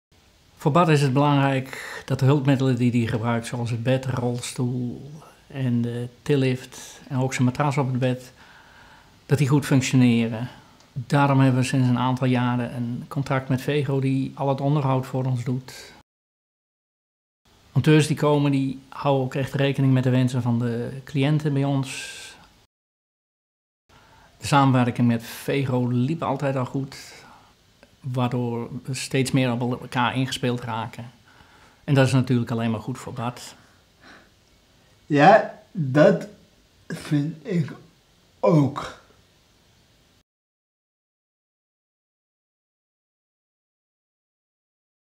Vegro Compleet - Service en onderhoud
Veiligheid staat voorop. Het is daarom belangrijk dat u uw hulpmiddelen goed onderhoudt en van tijd tot tijd laat keuren. Met Vegro compleet nemen wij u deze zaken graag uit handen. Wij kunnen al het onderhoud van uw producten coördineren. Dit is merkonafhankelijk en volgens de laatste richtlijnen. U heeft hiervoor slechts één aanspreekpunt nodig. Bovendien blijven de onderhoudskosten, het beheer en de staat van uw hulpmiddelen geheel inzichtelijk.